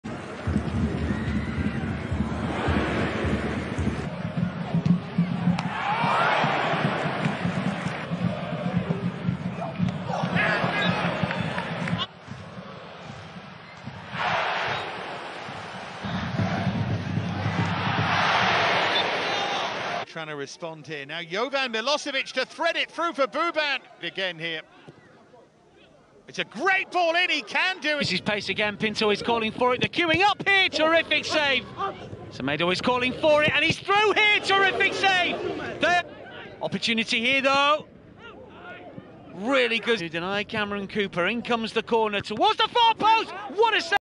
Theo Nicolini Sander | Denmark Goalkeeper | 2005 | Aalborg BK ᴴᴰ
Скаут «Манчестер Сити» присутствовал сегодня на товарищеском матче AaB, чтобы понаблюдать за 18-летним Тео Сандером в действии

GREEN TIME - Первый в YouTube канал о молодых и талантливых игроках.